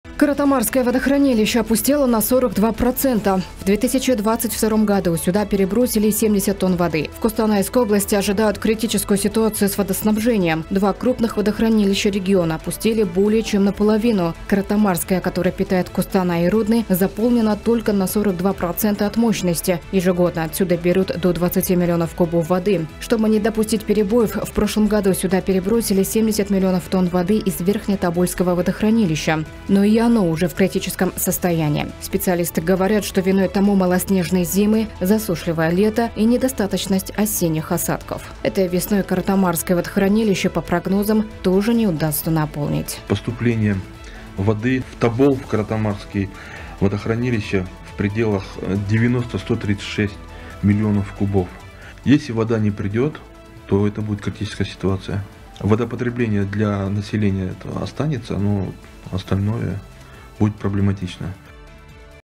Главные водохранилища костанайской области опустели на 60 %
В Костанайской области ожидают критическую ситуацию с водоснабжением. Два крупных водохранилища региона опустели более, чем наполовину. Каратомарское, которое питает Костанай и Рудный, заполнено только на 42 процента от мощности. Ежегодно отсюда берут до 20 млн кубов воды. Чтобы не допустить перебоев, в прошлом году сюда перебросили 70 млн тонн воды из Верхне-тобольского водохранилища. Но и оно уже в критическом состоянии. Специалисты говорят, что виной тому малоснежные зимы, засушливое лето и недостаточность осенних осадков. Этой весной Каратомарское водохранилище, по прогнозам, тоже не удастся наполнить.